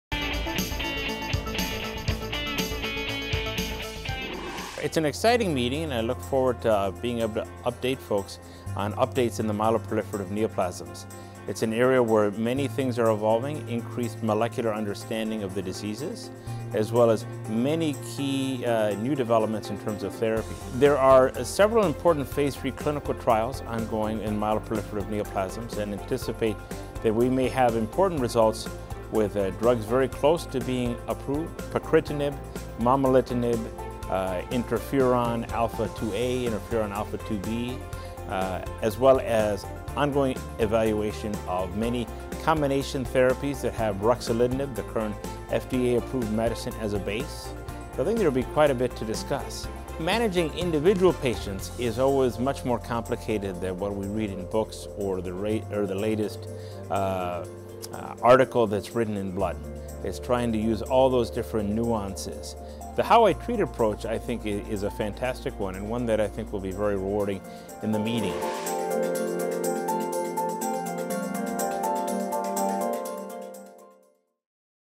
ASH Meeting on Hematologic Malignancies: Ruben Mesa, MD
ASH Meeting on Hematologic Malignancies speaker Ruben Mesa, MD, discusses the key clinical trials underway for new therapies for myeloproliferative neoplasms. Learn more about the ASH Meeting on Hematologic Malignancies happening September 17-19 in Chicago